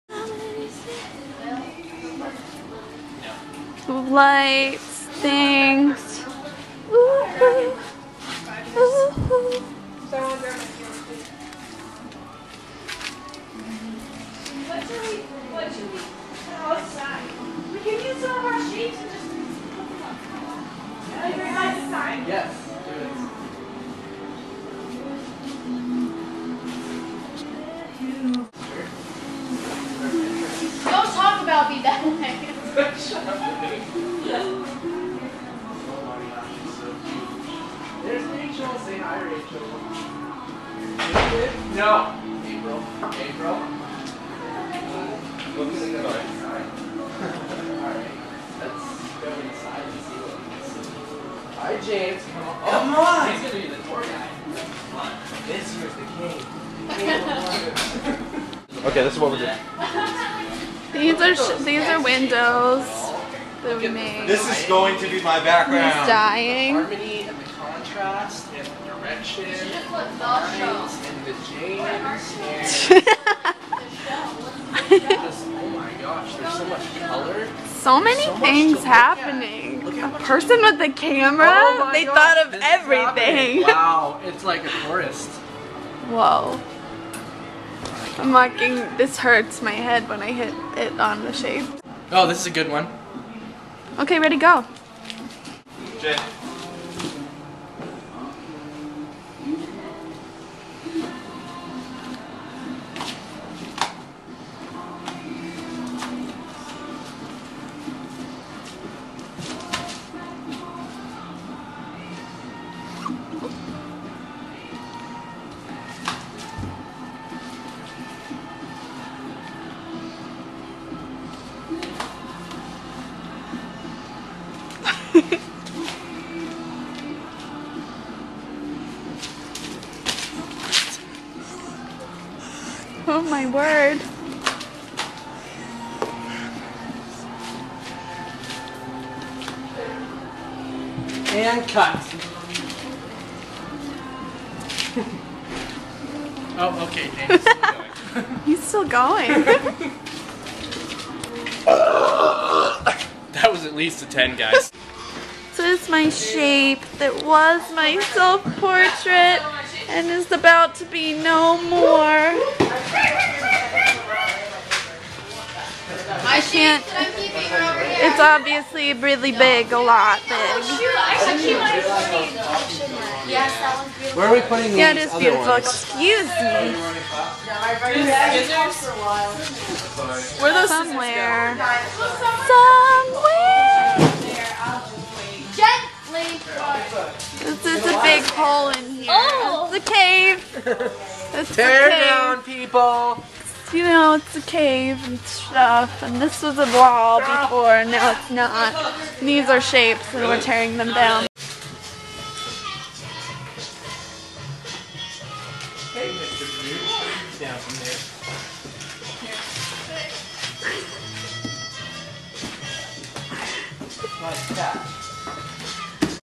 3D Design Group Project 2012
3-Dimensional Design Group Project 2012 at Mercyhurst University.
This project took about two weeks to create. We built all of the pieces individually and had one day to set them up in a 15' by 15' area in the studio.
The archway/entryway was reused. It used to be my personal project, a self portrait, and that took me about 60 hours to create. Of course it was repainted, it was raised up, and the back was cut out for this project. Saying goodbye was the saddest thing ever. But it was also fun because I got to be godzilla and stomp all over it. But also very sad.

J Clement and James Conley are starring in the "music video" portion. We decided the project was definitely perfect for music videos. Yes. (J teaches dance... you can tell...)

Artists: Kimberlyn Bloise, April Alfieri, J Clement, James Conley, Anna Cooney, Brittany Drabert, Kelsey Elliott, and Rachel Clark